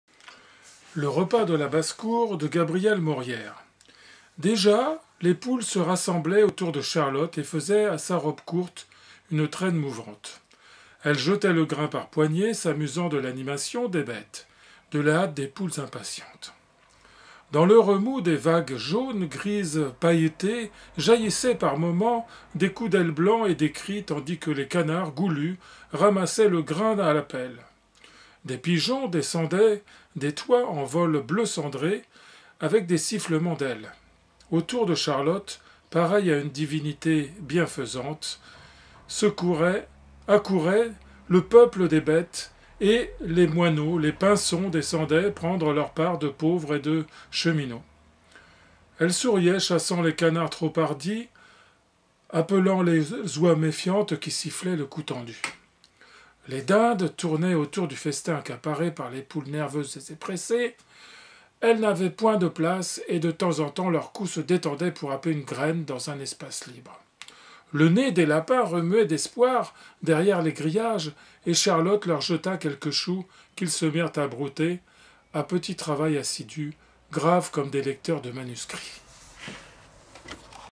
Le repas de la basse cour (Gabriel Maurière)
remous : mouvement de l'au  qui se meut en arrière en rencontrant un obstacle. Et dans le texte ?
paillettes : brillants comme des paillettes (petites pailles) ou lames minces de métal ou de verre qu'on applique sur une étoffe.
méfiant : qui n'a pas confiance (rapprocher foi, fidèle, se fier, se défier, etc...accaparé : pris pour soi au détriment des autres.
assidu : (même famille que asseoir et siège) ; qui se tient auprès, continu, constant.
Un tableau vivant de la basse cour au moment du repas. Etudiez les traits qui peignent les attitudes et les mouvements de chaque bête (les poules, les pigeons, les moineaux, les canards, les oies, les dindes, les lapins...)
Quelques expressions particulièrement heureuse : les poules faisant à sa robe une traîne mouvante..., des coups d'aile blancs..., leur part de chemineaux..., les lapins qui broutent.